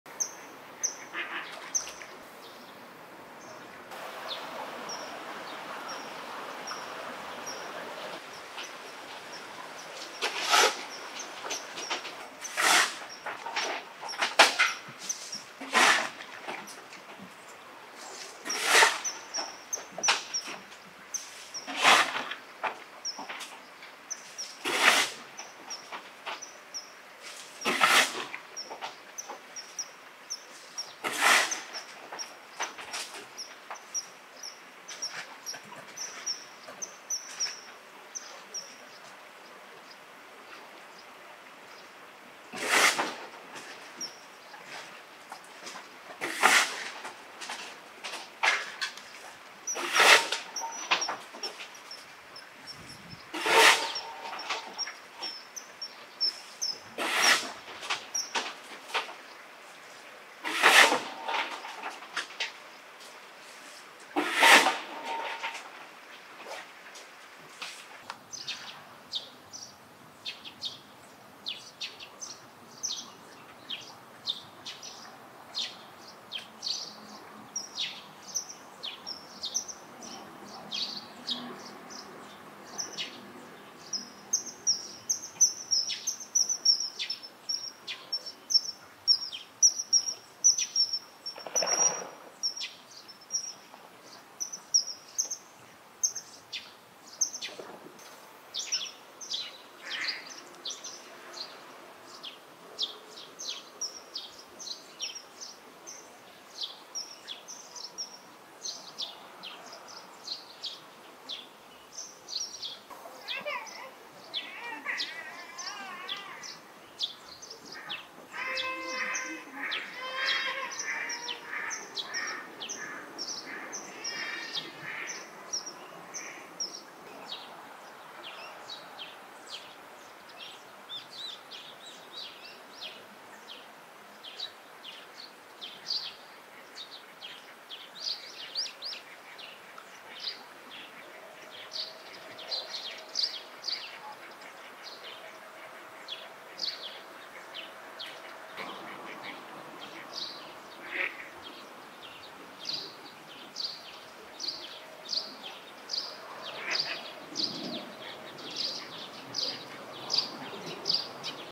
珍しい鳥の瞬間。カワセミの求愛給餌でホバリング、交尾失敗
交尾がしたくて求愛給餌のプレゼントを頑張ったカワセミの雄だが…肝心のタイミングで逃げられてしまった。でも、夫婦っぽいよ❤